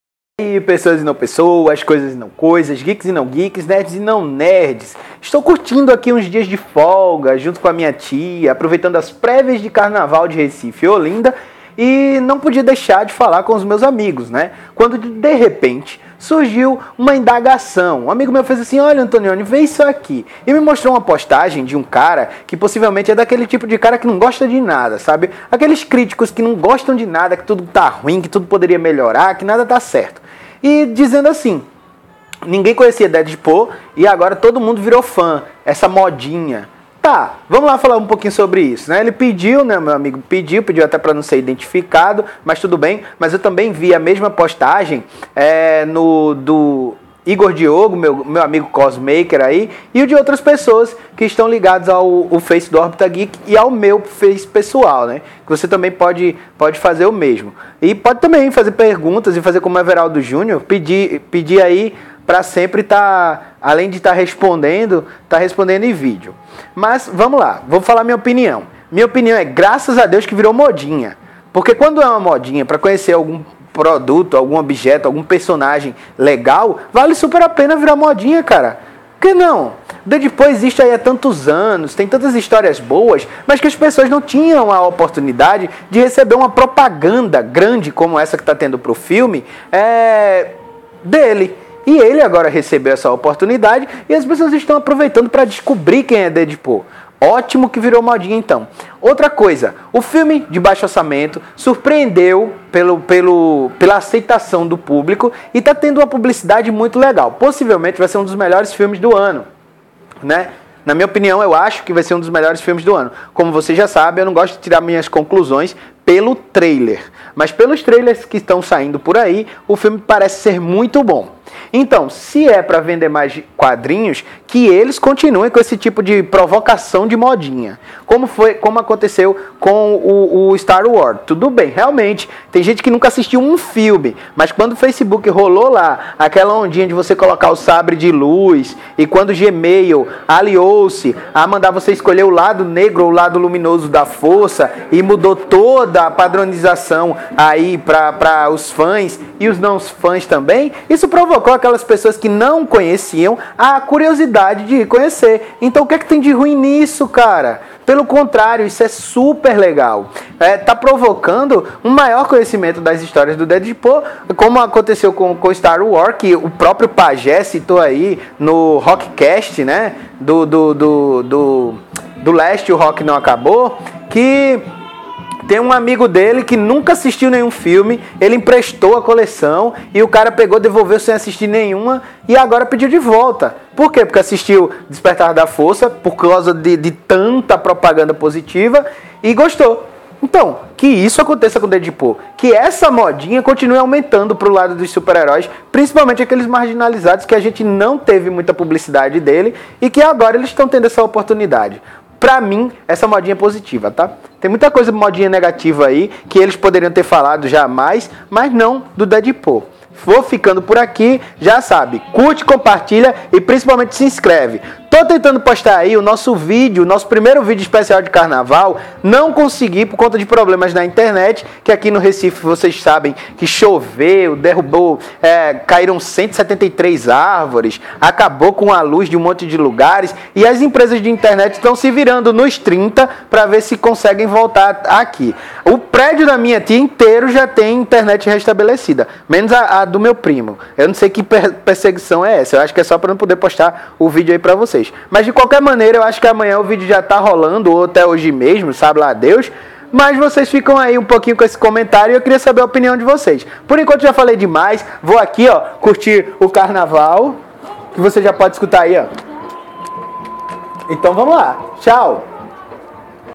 DEADPOOL EM CONVERSA DE FIM DE SEMANA
Em uma conversa de fim de semana Deadpool foi sitado como modinha ... ixi!